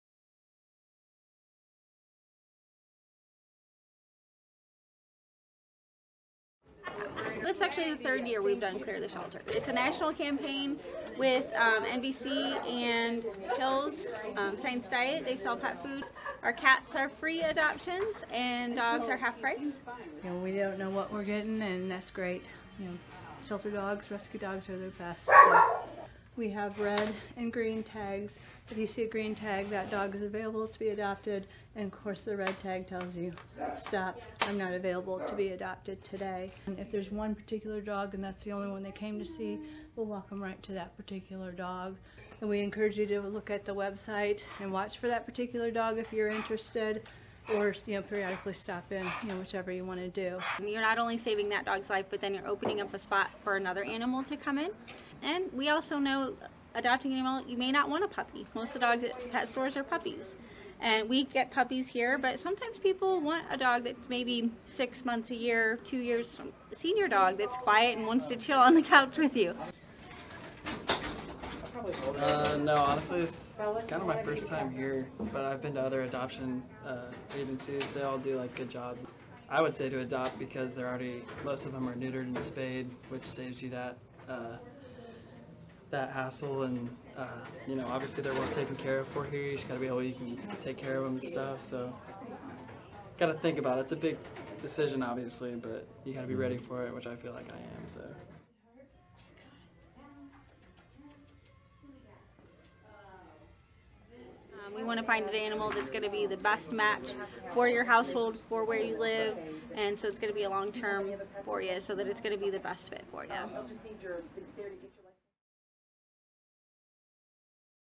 'Clear The Shelters' In Fort Wayne
Take a look inside the Allen County SPCA's "Clear The Shelter" event, held on Saturday, August 18, 2018, to see why you should consider adopting your next pet.

"Clear The Shelters" is a nationwide pet adoption drive that has helped nearly 250,000 pets find homes since 2015, with over 90,000 adoptions just this year. In 2018, over 1,200 shelters across the country teamed up with NBC to “Clear the Shelters”.